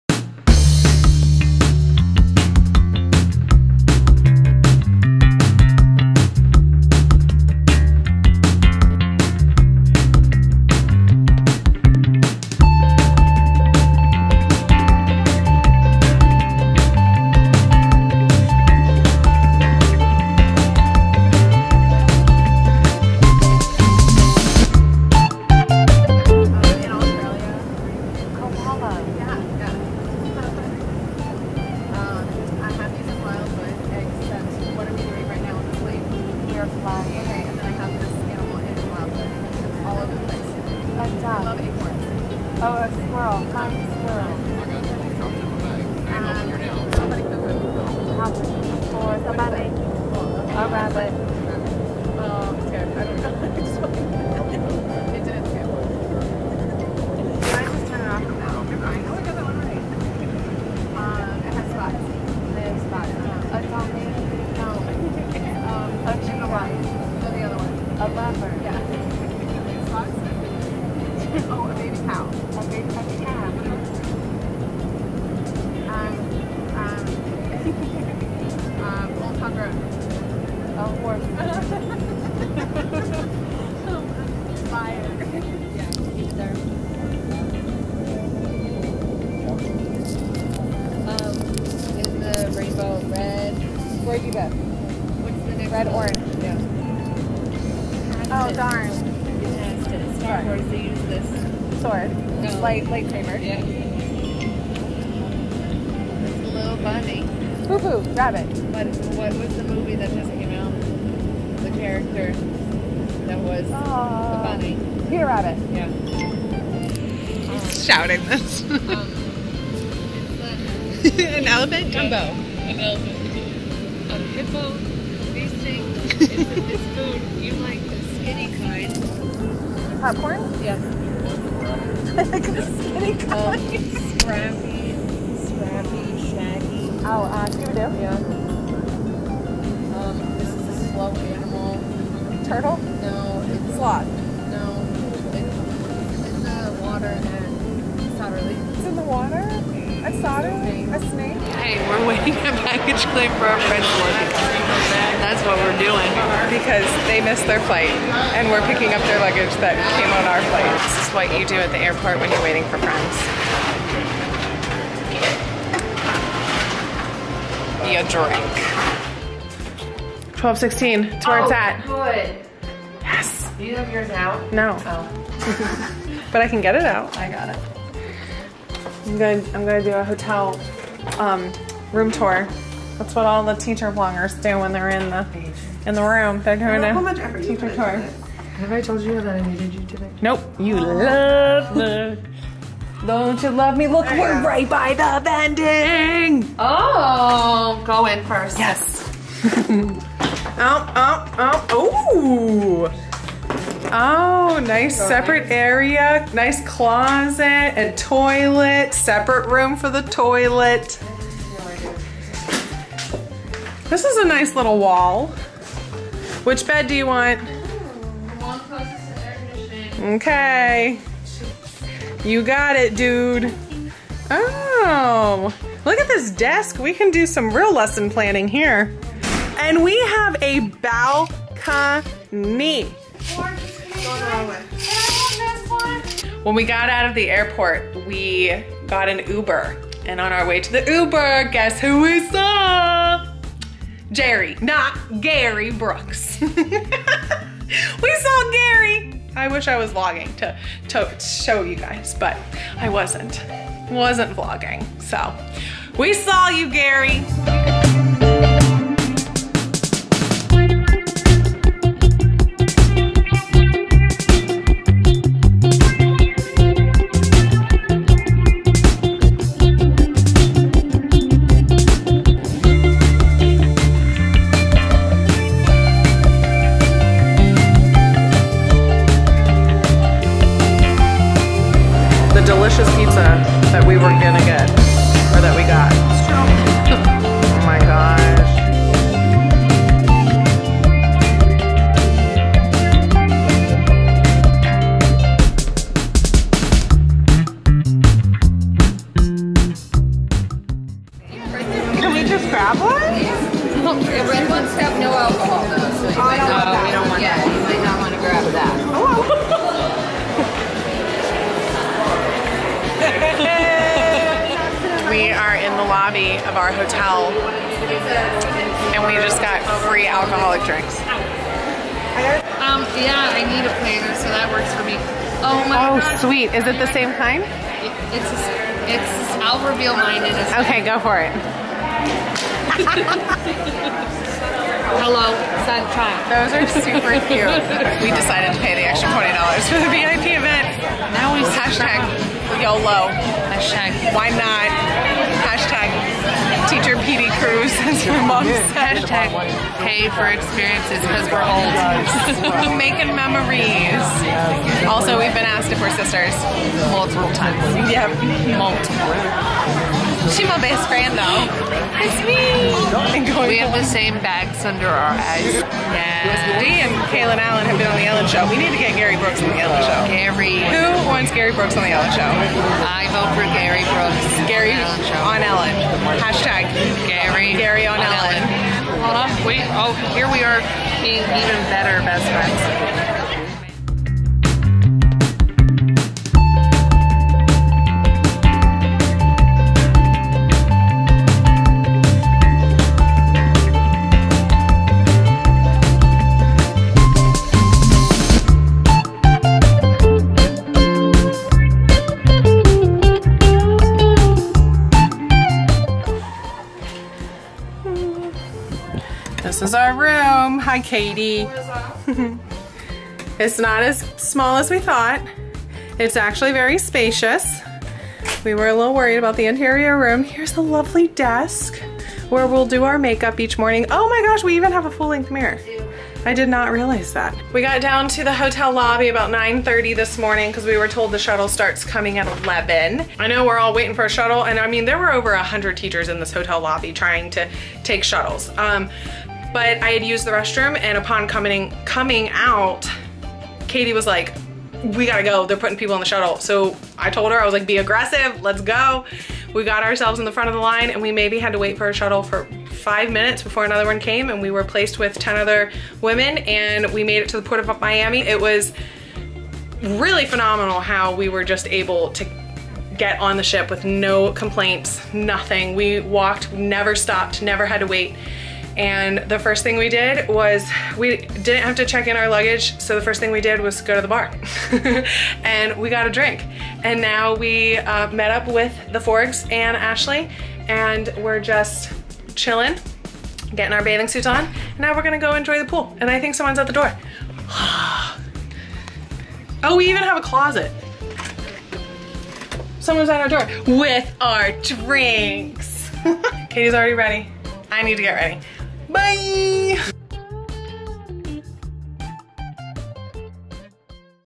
PEP TALK IN MIAMI | TEACH YOUR HEART OUT | BAHAMAS CRUISE | TEACHER VLOG
After watching a few teacher vlogs, I became inspired to create my own.